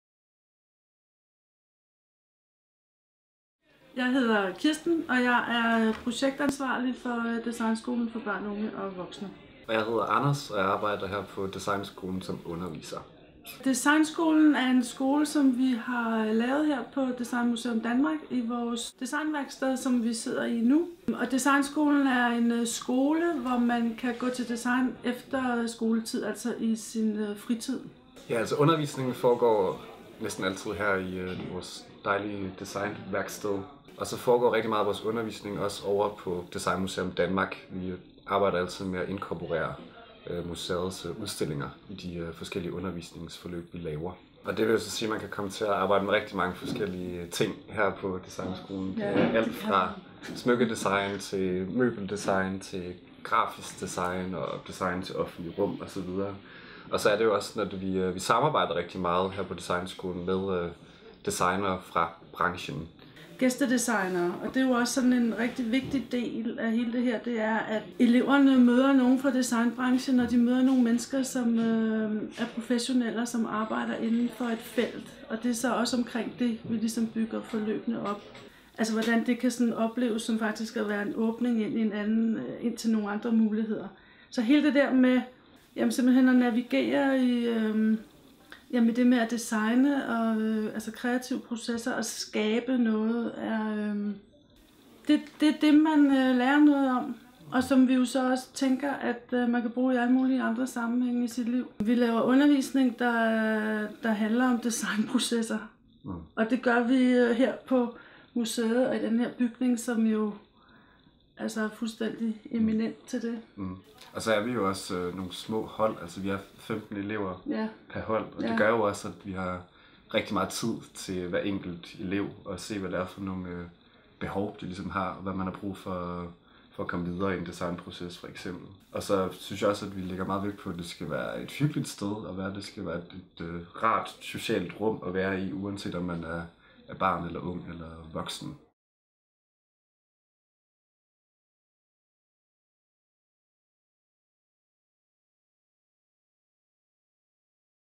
Designmuseum Danmarks Designskole
Designskolen på Designmuseum Danmark tilbyder en række forskellige hold og aktiviteter for både børn, unge og voksne. Det hele foregår i museets Designværksted, der med sine velproportionerede og smukke arkitektoniske rammer mulighed for at arbejde kreativt med design under helt optimale forhold.